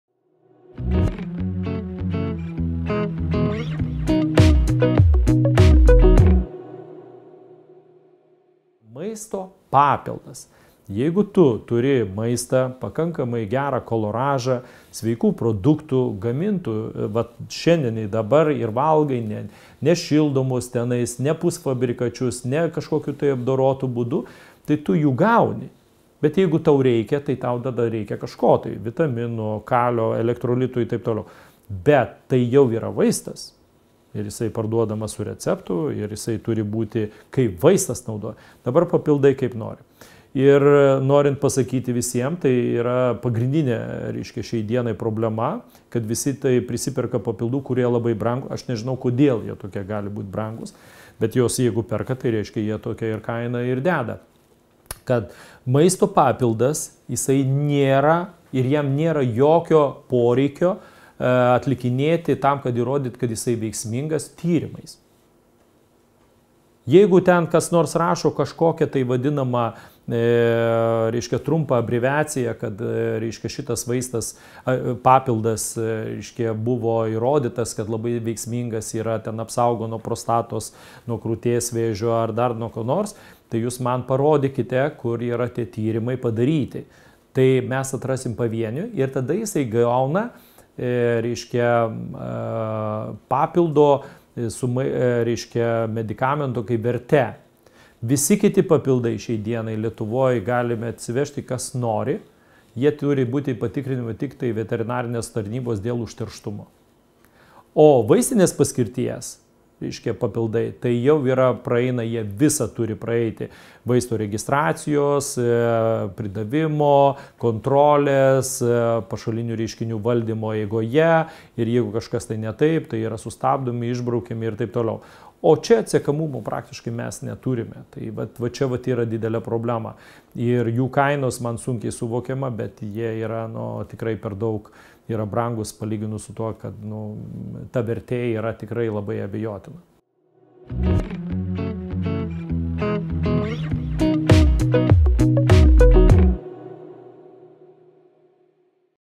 Vartojate papildus? Sužinokite apie jų keliamus pavojus
Kuo skiriasi įprasti papildai nuo vaistinės paskirties papildų? Ar tikrai vartodami papildus mes geriname sveikatą?

Apie papildų vartojimo pavojus ir problemas paaiškina gydytojas onkologas chemoterapeutas Alvydas Česas.